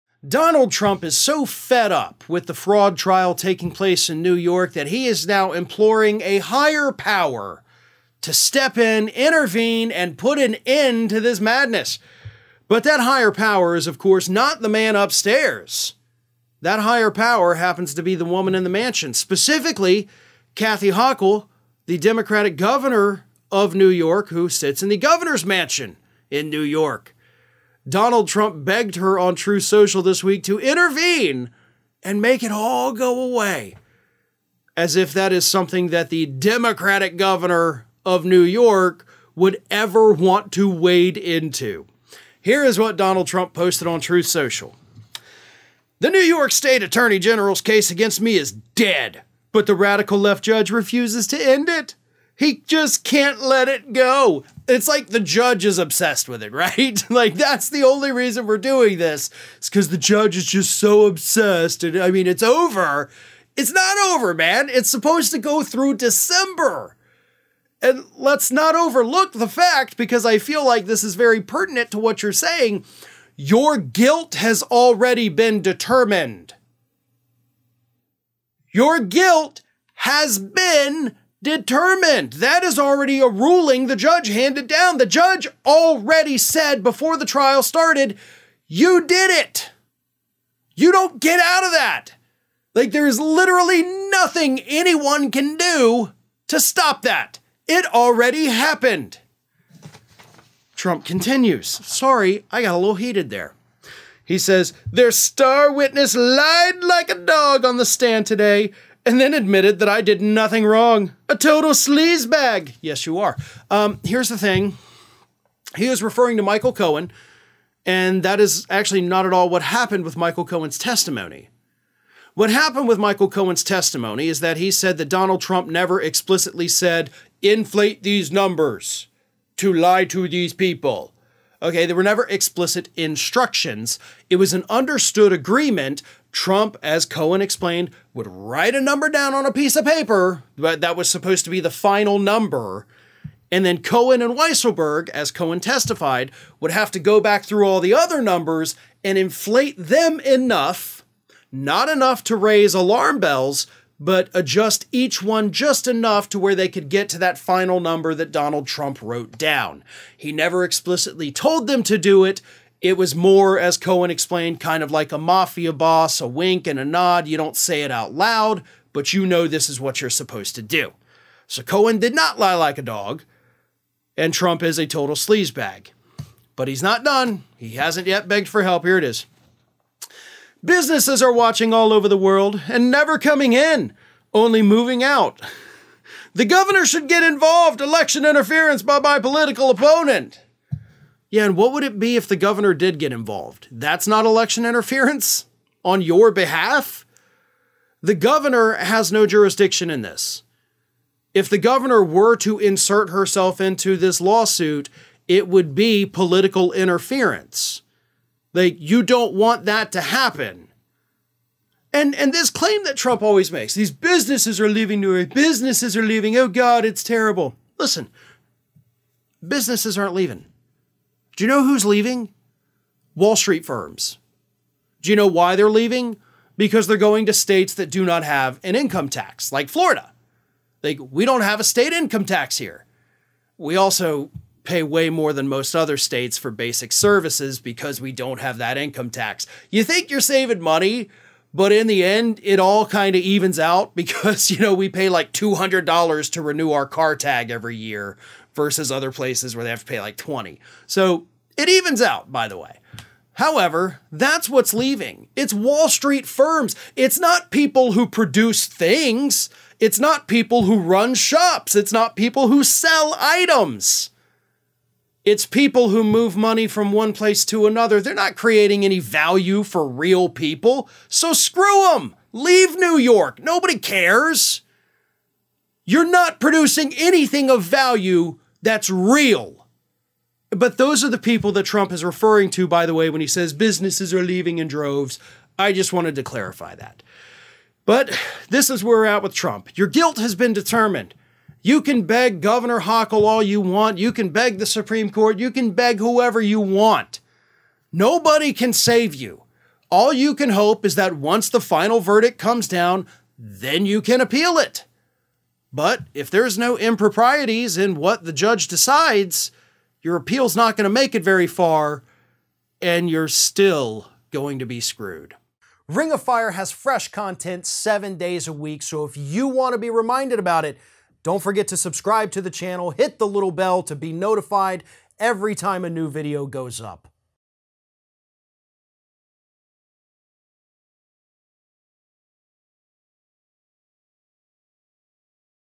Trump Begs New York Governor To Save Him From Fraud Trial
An angry and desperate Donald Trump took to Truth Social this week to beg for intervention in the ongoing fraud trial that he's involved in. Trump went as far as to beg New York's Democratic governor Kathy Hochul to intervene while also saying that Michael Cohen "lied like a dog" on the stand. But not only did Cohen NOT exonerate Trump, the governor would be abusing her power if she were to get involved. Ring of Fire's Farron Cousins explains what's happening.

Donald Trump is so fed up with the fraud trial taking place in New York, that he is now imploring a higher power to step in, intervene, and put an end to this madness. But that higher power is of course not the man upstairs. That higher power happens to be the woman in the mansion, specifically Kath Hle, the Democratic governor of New York, who sits in the governor's mansion in New York. Donald Trump begged her on true social this week to intervene and make it all go away, as if that is something that the democratic governor of New York would ever want to wade into. Here is what Donald Trump posted on truth social. The New York State Attorney General's case against me is dead. But the radical left judge refuses to end it. He just can't let it go. It's like the judge is obsessed with it, right? like, that's the only reason we're doing this. It's 'cause the judge is just so obsessed, and I mean, it's over. It's not over, man. It's supposed to go through December. And let's not overlook the fact, because I feel like this is very pertinent to what you're saying. You guilt has already been determined.

Your guilt has been determined. That is already a ruling. The judge handed down, the judge already said before the trial started, you did it. You don't get out of that. Like there is literally nothing anyone can do to stop that. It already happened. Trump continues. Sorry, I got a little heated there. He says their star witness lied like a dog on the stand today and then admitted that I did nothing wrong. A total sleaze bag. Yes, you are. Um, here's the thing. He was referring to Michael Cohen, and that is actually not at all what happened with Michael Cohen's testimony. What happened with Michael Cohen's testimony is that he said that Donald Trump never explicitly said, inflate these numbers to lie to these people. Okay? They were never explicit instructions. It was an understood agreement. Trump, as Cohen explained, would write a number down on a piece of paper, but that was supposed to be the final number.

And then Cohen and Weisselberg, as Cohen testified, would have to go back through all the other numbers and inflate them enough, not enough to raise alarm bells, but adjust each one just enough to where they could get to that final number that Donald Trump wrote down. He never explicitly told them to do it. It was more as Cohen explained, kind of like a mafiaa boss, a wink, and a nod. You don't say it out loud, but you know this is what you're supposed to do. So Cohen did not lie like a dog. And Trump is a total sleazebag, but he's not done. He hasn't yet begged for help. Here it is.

Businesses are watching all over the world and never coming in, only moving out. The governor should get involved. Election interference by my political opponent. Yeah. And what would it be if the governor did get involved? That's not election interference on your behalf. The governor has no jurisdiction in this. If the governor were to insert herself into this lawsuit, it would be political interference. Like you don't want that to happen. And, and this claim that Trump always makes, these businesses are leaving you. A businesses are leaving. Oh God, it's terrible. Listen, businesses aren't leaving. Do you know who's leaving? Wall Street firms. Do you know why they're leaving? Because they're going to states that do not have an income tax. Like Florida, like, we don't have a state income tax here.